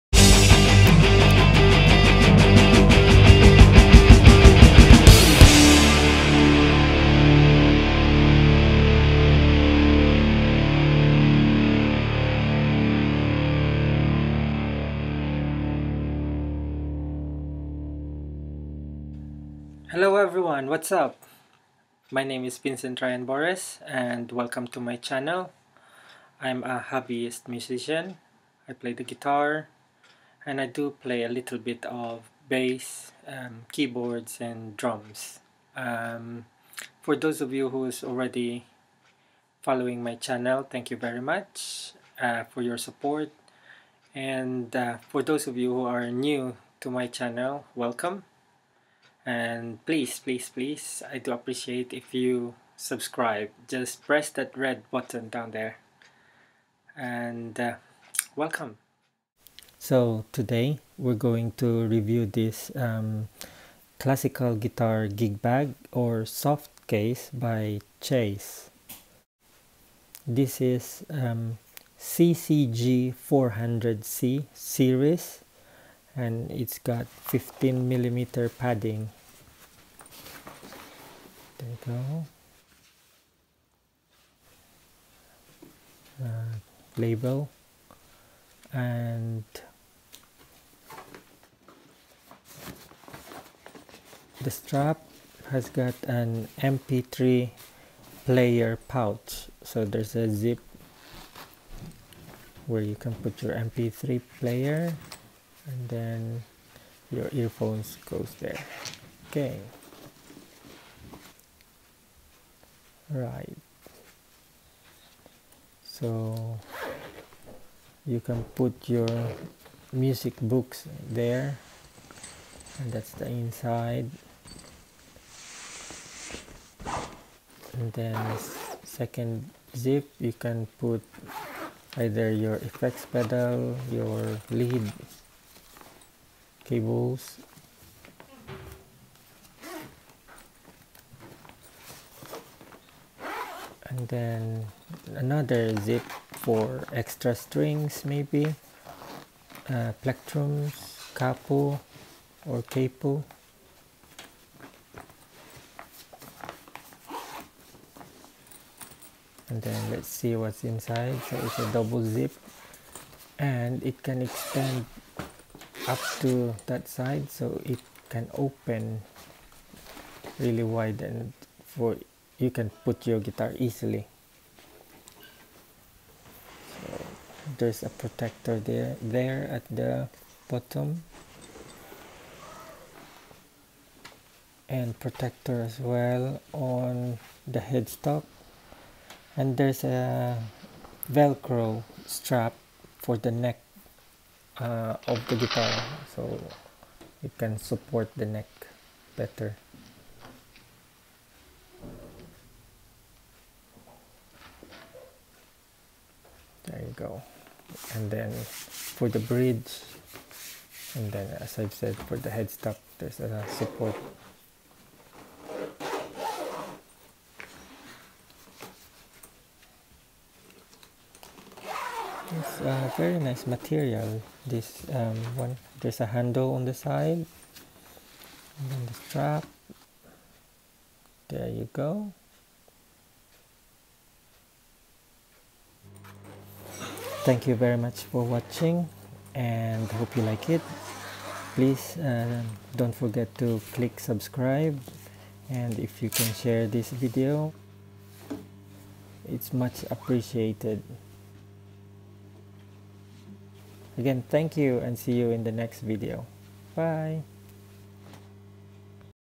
Review of Chase CCG400C Classical Nylon Guitar Gigbag
Classical guitar gigbag softcase CCG400C by Chase. 15 mm padding. I bought this for my Valencia vc104ebk electro classical guitar.